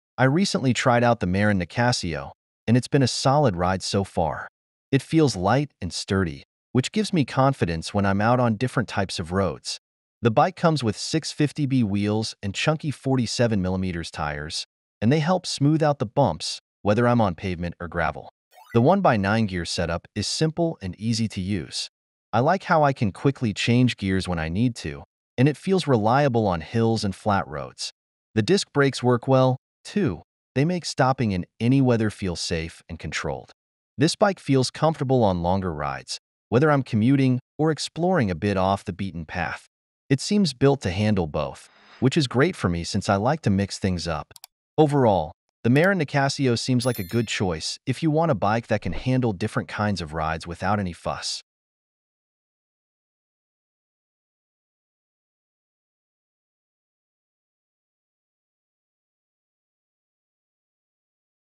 Why the Marin Nicasio Might Be the Only Bike You’ll Ever Need
I took the Marin Nicasio for a test ride to see how it handles different terrains and conditions. From smooth roads to gravel paths, this bike impressed me with its versatility, comfort, and reliable performance. Here’s what you need to know about the Marin Nicasio.

00:00 Smooth Ride on Any Terrain
00:18 Simple Gear Setup and Reliable Brakes
00:33 Comfortable for Long Rides
00:43 Versatile Choice for All Riders